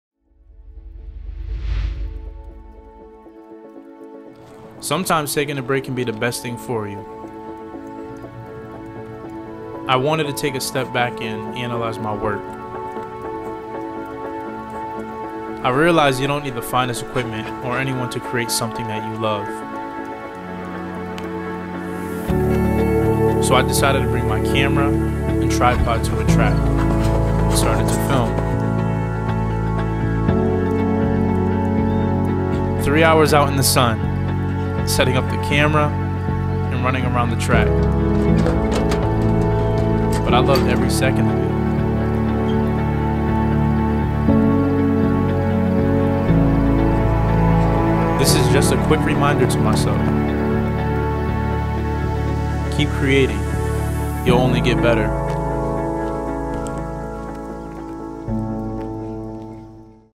Nike Spec Commercial We Created (Motivation)
Just Create! That is the motto we follow here at Out The Box Digital Marketing. Live full and die empty, you do not want to leave this planet without giving everything out of yourself first.

This is a Nike commercial that we created to let you know not to hold back! Create and let the world see your work!